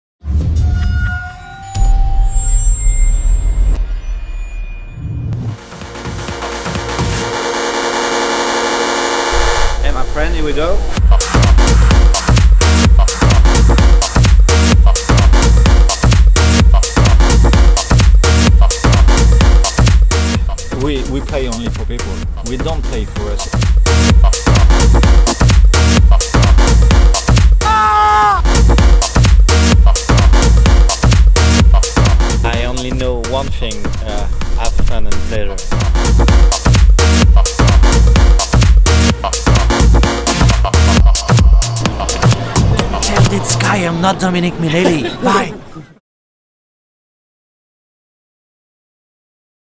Passion Of Love - Dominic Minelli @ base music club
24 kwietnia w Base Music Clubie odbyła się już kolejna edycja Passion Of Love, na której zagościł znany ze swojego show na planecie.fm  DOMINIC MINELLI. Nasza kamera towarzyszyła mu od samego przybycia na lotnisko aż do zakończenia imprezy.

Już dziś zapraszamy wszystkich użytkowników naszego video portalu do obejrzenia premierowego zwiastunu, który jest przedsmakiem tego co zobaczycie na oficjalnym filmie - premiera już niebawem!